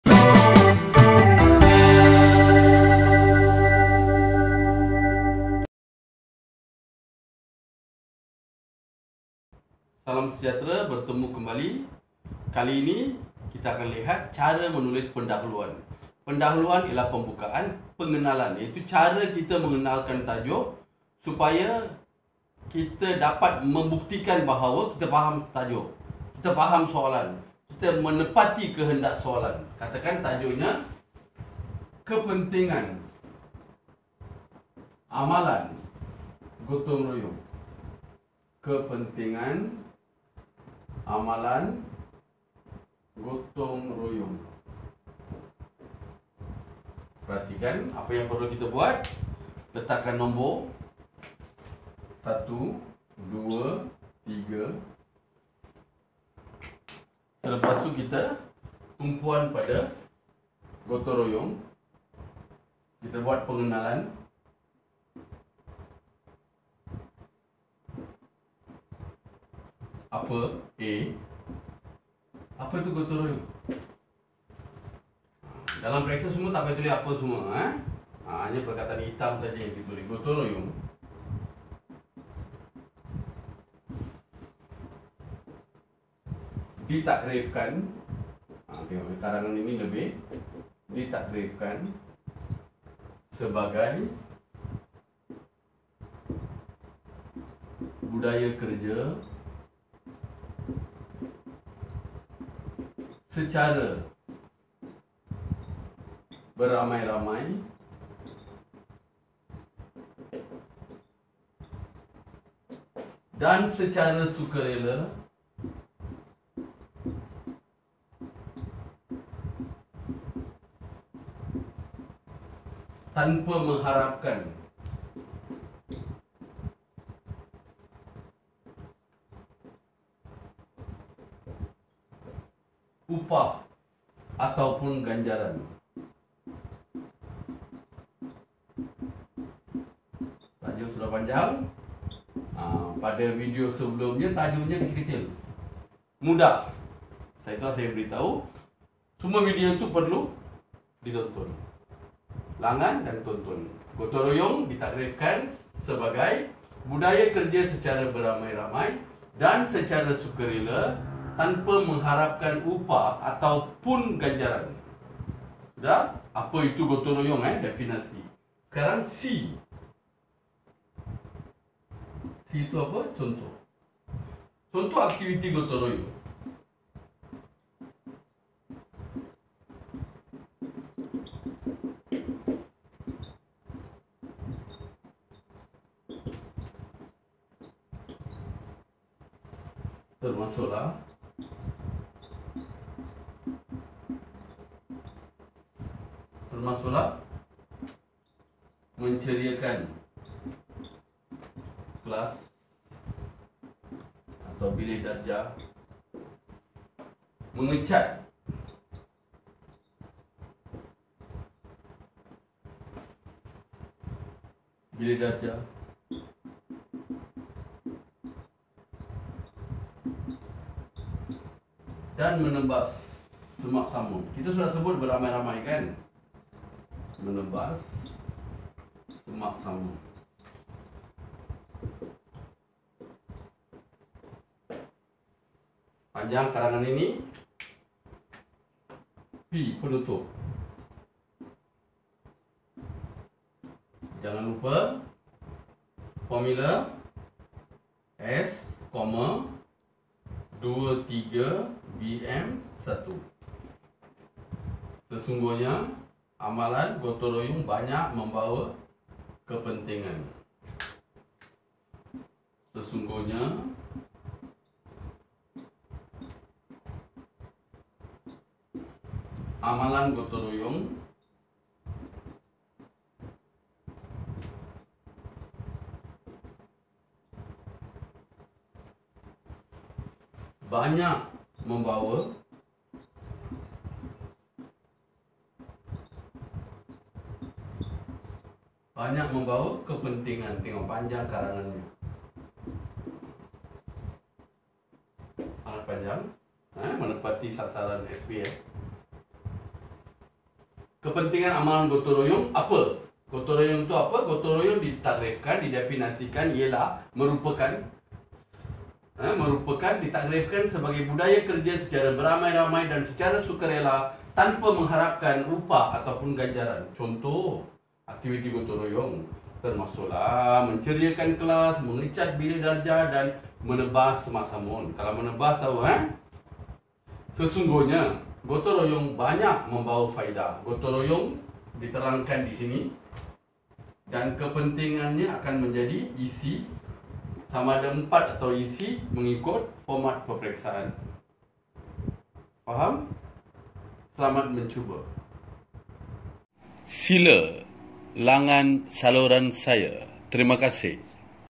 SIRI 17: TEKNIK PENGENALAN KARANGAN (KEPENTINGAN AMALAN GOTONG-ROYONG)
CARA MENULIS PENDAHULUAN KARANGAN SECARA BERKESAN DAN MUDAH. SESUAI BUAT PELAJAR SEKOLAH RENDAH DAN PELAJAR SEKOLAH MENENGAH